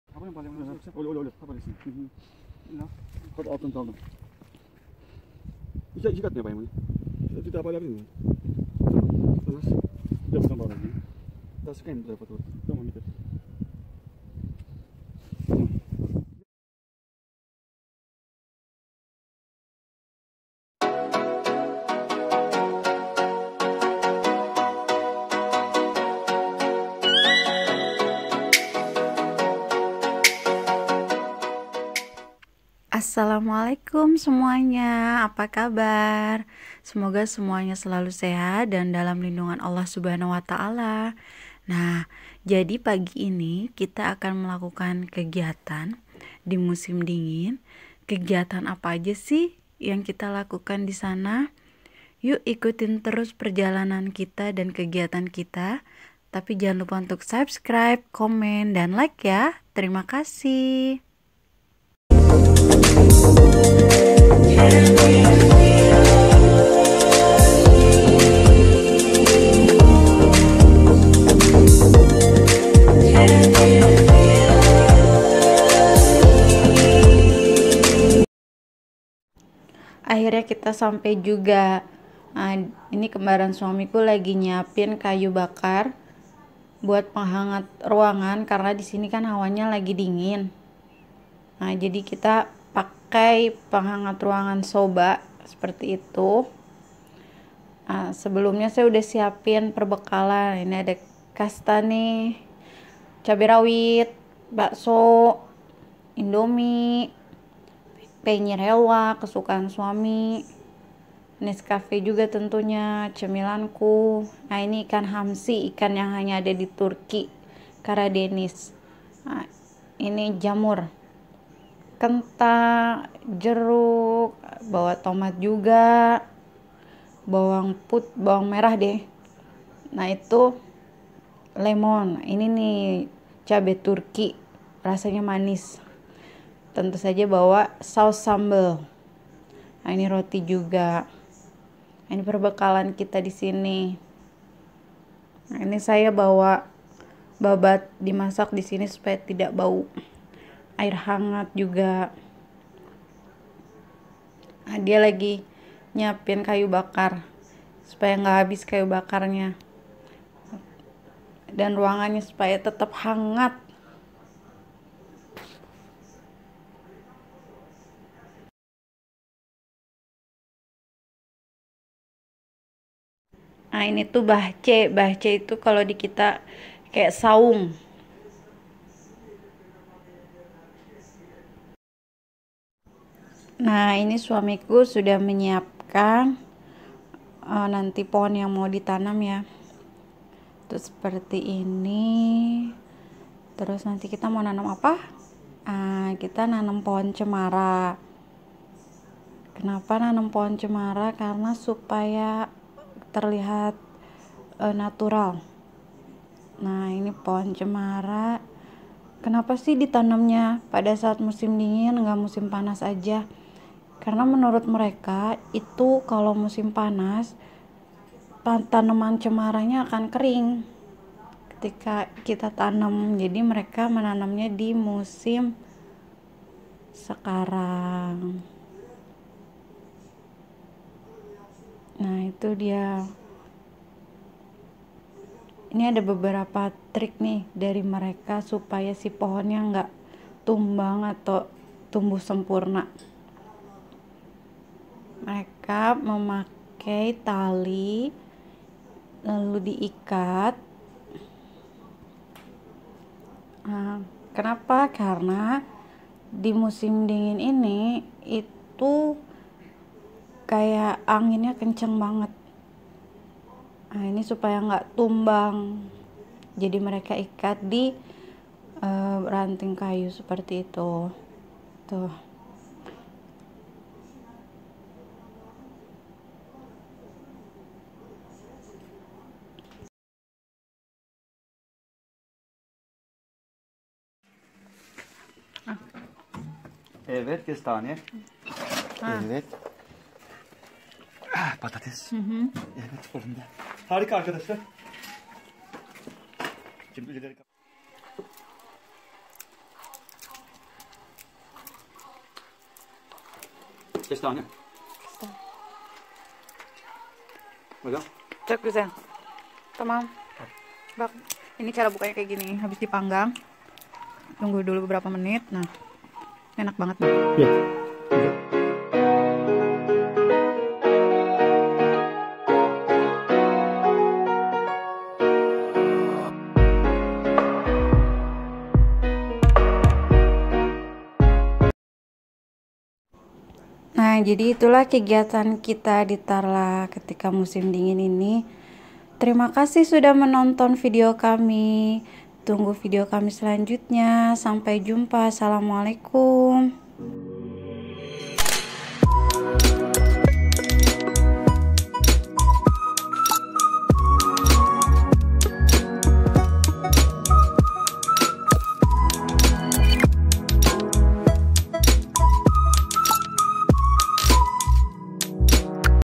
NANEM POHON CEMARA DI MUSIM DINGIN!!! DAN MENIKMATI MAKANAN ALA TURKI DI LADANG
istagram : Najahuseyin44